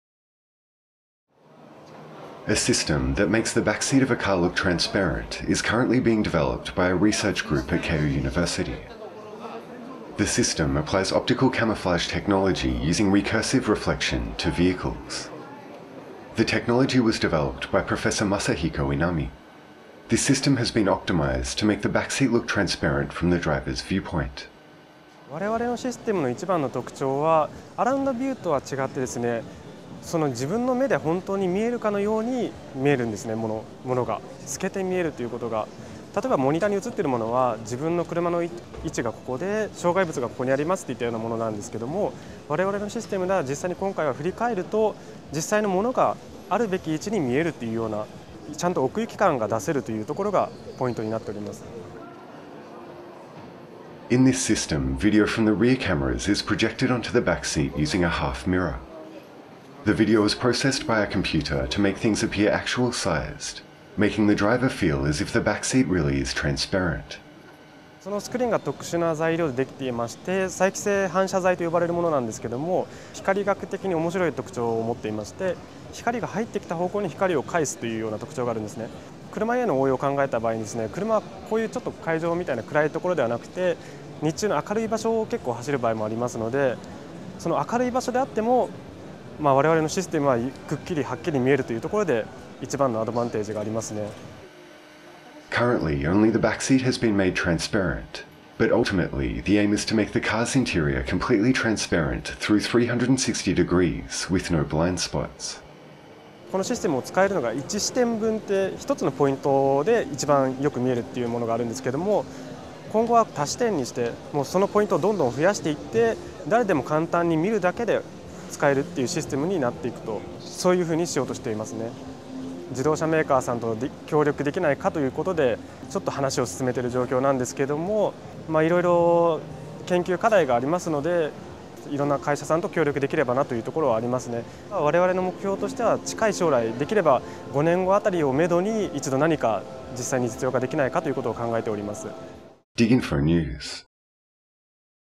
Optical camouflage tech removes backseat of a car, transparent interior the goal
25/10/2012 Digital Contents Expo 2012

Keio University
Transparent Prius